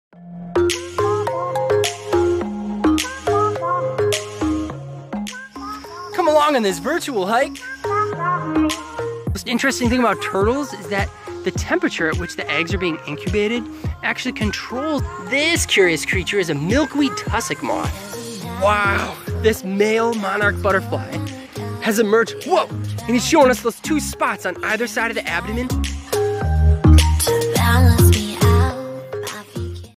Virtual Hikes (Trailer)
Nature documentaries for curious minds of all ages and backgrounds. Videos feature trailside wildlife including dragonflies, trees, wildflowers, turtles, and more!

All opinions or views expressed are my own and not the position of Cleveland Metroparks.